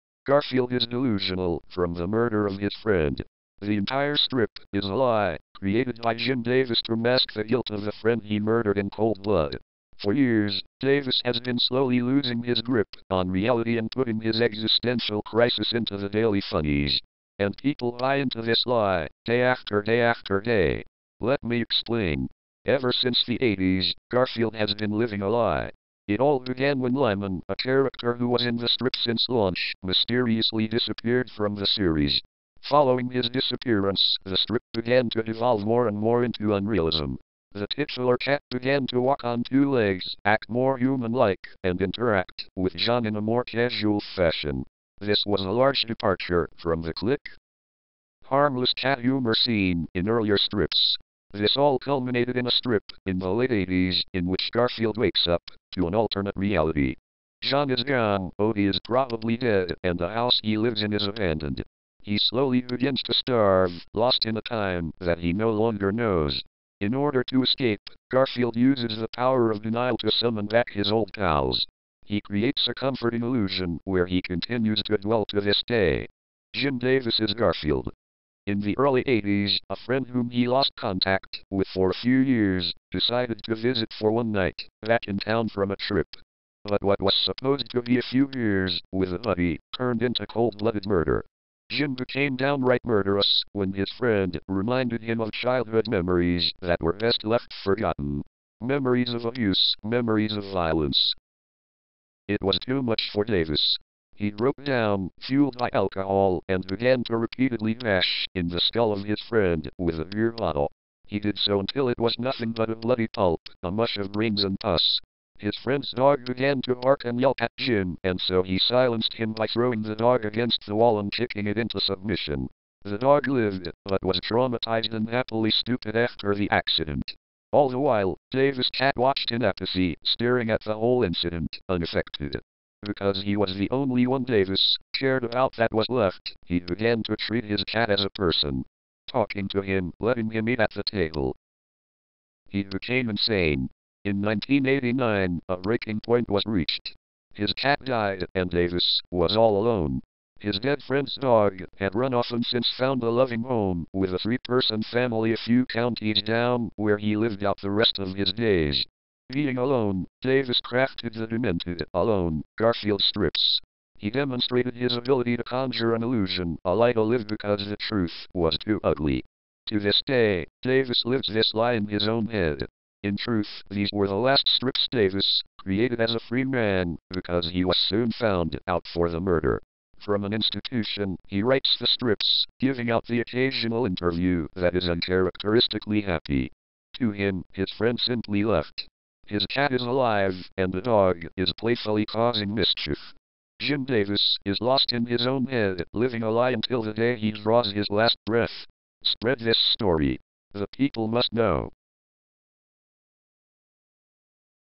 Creepypasta Garfield is a lie Speakonia Version
Here is another speakonia creepypasta, this one is about garfield